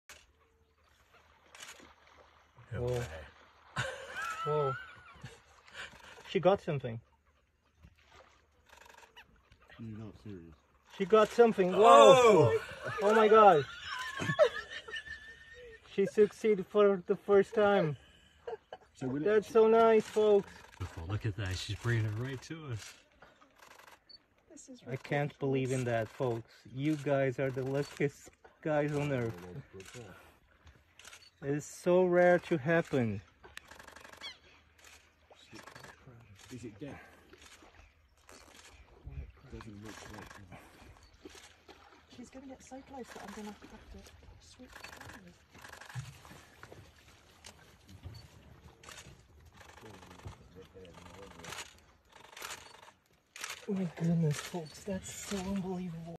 🐆vs🐊 Jaguar catching caiman at Pantanal / Onça pega jacaré no Pantanal
Demorou 14 anos trabalhando como guia de ecoturismo para conseguir capturar esse momento! Uma onça-pintada pegando um jacaré a menos de 4 metros do nosso carro! Que dia! Que cena! Que fantástico! Obrigado @k_zelnak1 por capturar o momento da foto em vídeo!

It took 14 years working as an ecotourism guide to capture this moment! A jaguar catching a caiman less than 4 meters from our car! What a day! What a scene! It was awesome! Thanks @k_zelnak1 for capturing the photo moment on video!